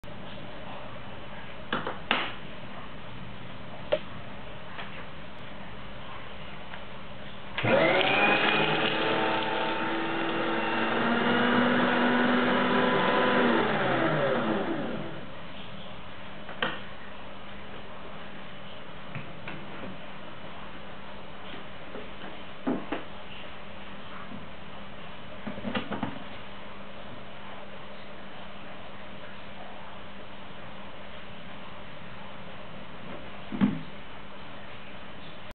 Mesin Penghancur Kertas Paper Shredder SECURE EzSC-10A Cross Cut
Mesin Penghancur Kertas Paper Shredder SECURE EzSC-10A Cross Cut, HOT PRODUCT, Best Seller. Sangat mudah pengoperasiannya, Garansi Resmi 1 tahun, bisa Delivery utk daerah Jakarta, Cash On Delivery (C.O.D). Jakarta Only.